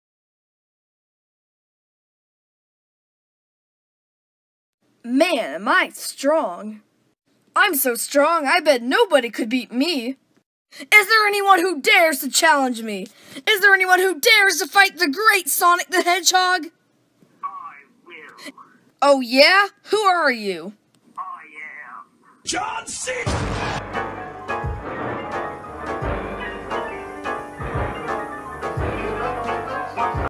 Animatic - Sonic Meets His Match
This was a comic that I drew as a rough draft for the Sonic Meets His Match animation back in the day. The draft itself was just a comic written on a single sheet of paper; I just decided to take those drawings and put together an “animatic” of it so that I could show it off.